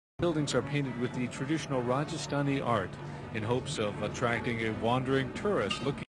International Film & Video
A traditional Rajasthani painting on a wall depicts a rider on a rearing horse.